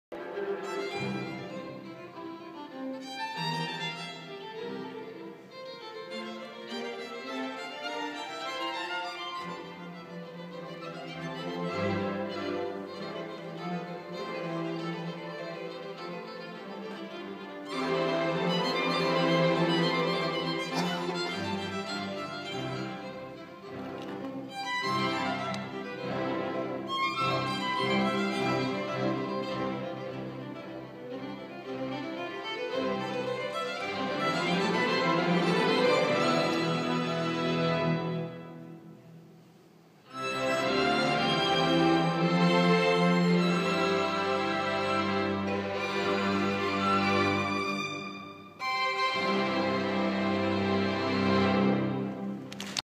Pekka Kuusisto, Fritz Kreisler
Moscow recital, Feb 2015, Misica Viva , Alexander Rudin